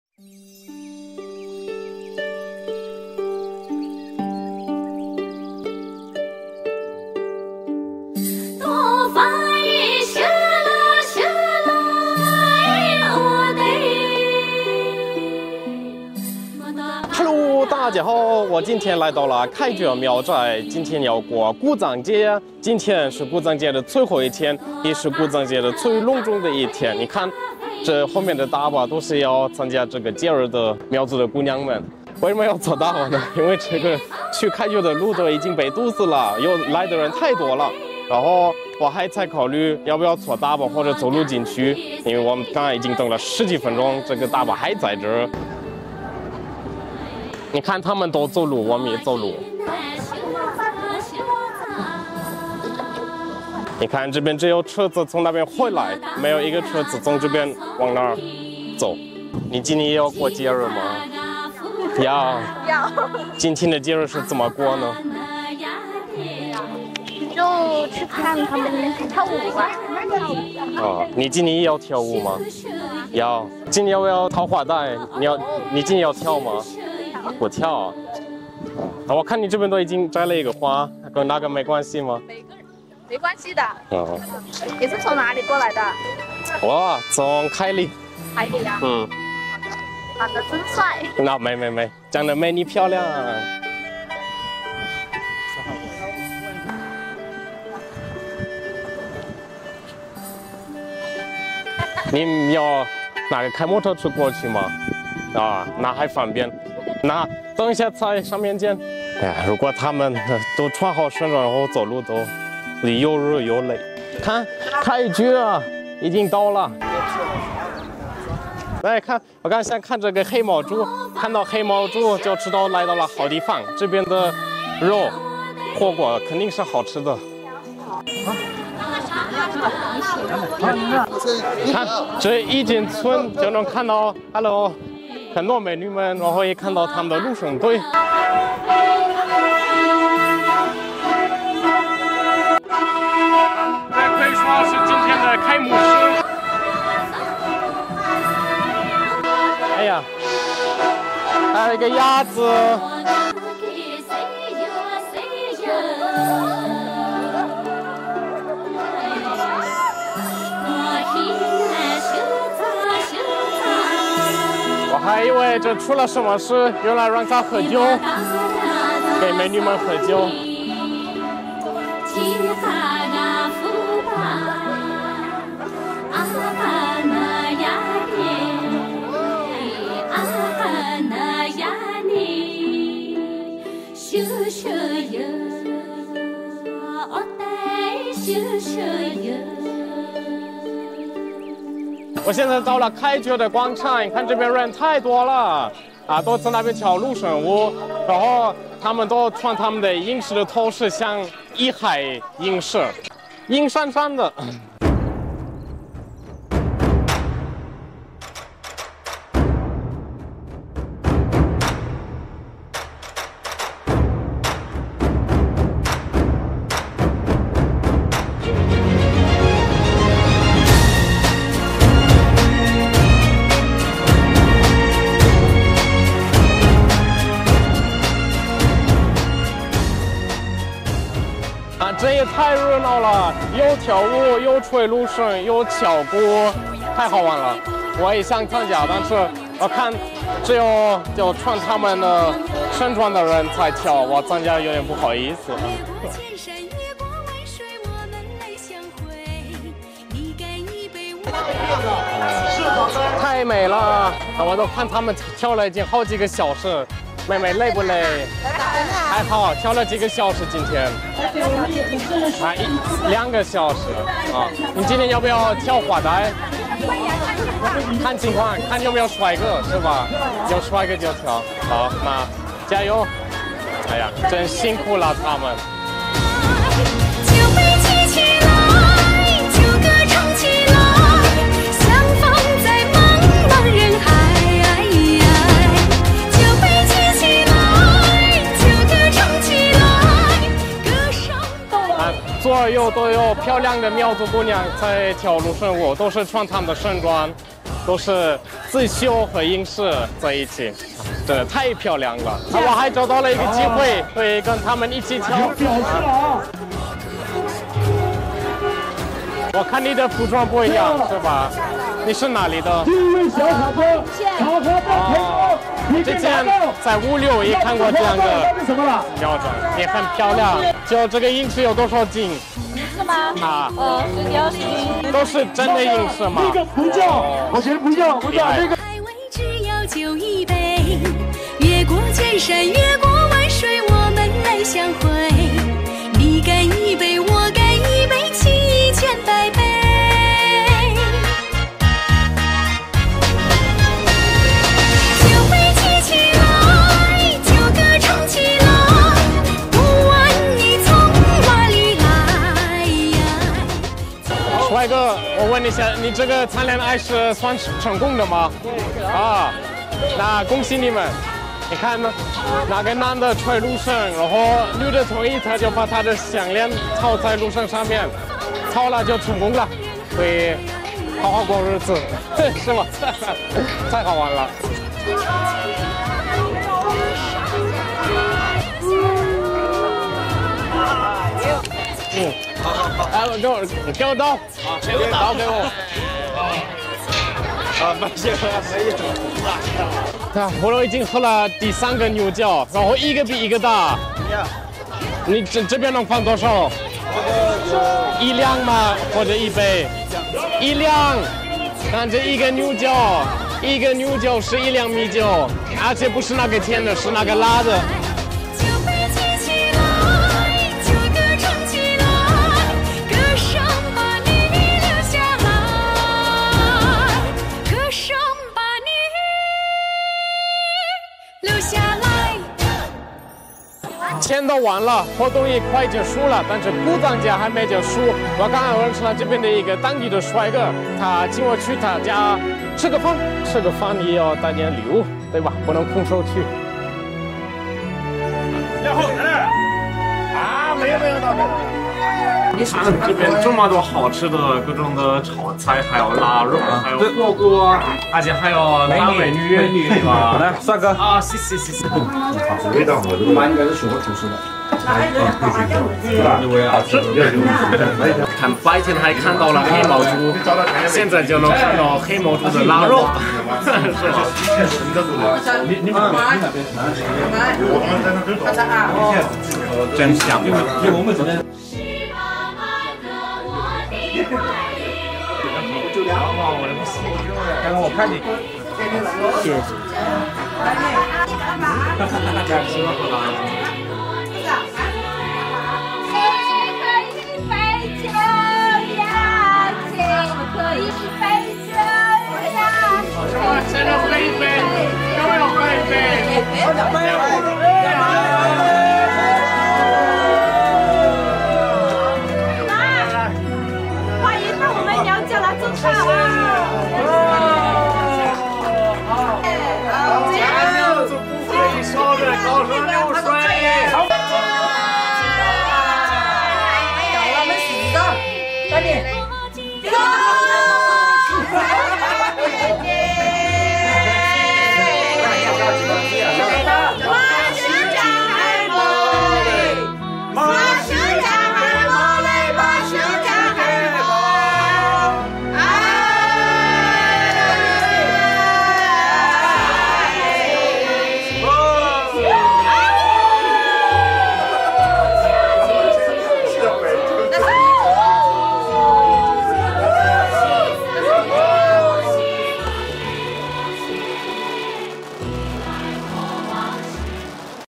You can only attend this Festival 7 times in your Life! 开觉鼓藏节 跟苗族姑娘跳芦笙舞喝高山流水
🐉Kuaishou 快手 (2 Million subs)